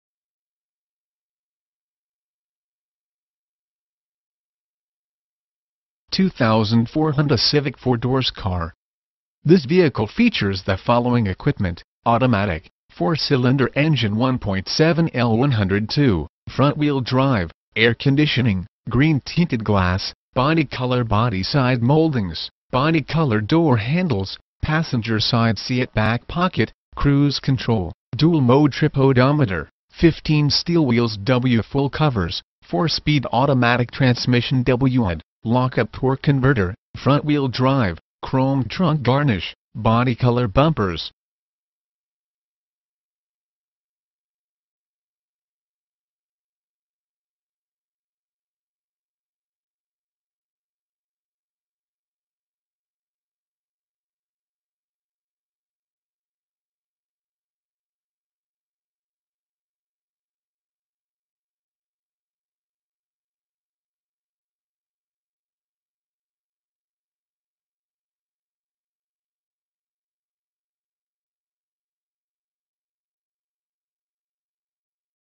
2004 Honda Civic - Premier Subaru KIA - Branford, CT 06405
Premier Subaru KIA
150-165 North Main Street

For Sale: 2004 Honda Civic
VIN: 2HGES16504H564758
Engine: 4 Cylinder Engine 1.7L/102
Drivetrain: Front Wheel Drive
Transmission: Automatic
Mileage: 99,260
Color: Eternal Blue Pearl (exterior) Gray (interior)

Features:
Two Thousand Four Honda Civic four doors Car. This vehicle features the following equipment: Automatic, 4 Cylinder Engine 1.7L/102, Front Wheel Drive, AIR CONDITIONING, Green-tinted glass, Body-color body-side moldings, Body-color door handles, Passenger side seatback pocket, Cruise control, Dual mode trip odometer, 15" steel wheels w/full covers, 4-speed automatic transmission w/OD, lockup torque converter, Front wheel drive, Chrome trunk garnish, Body-color bumpers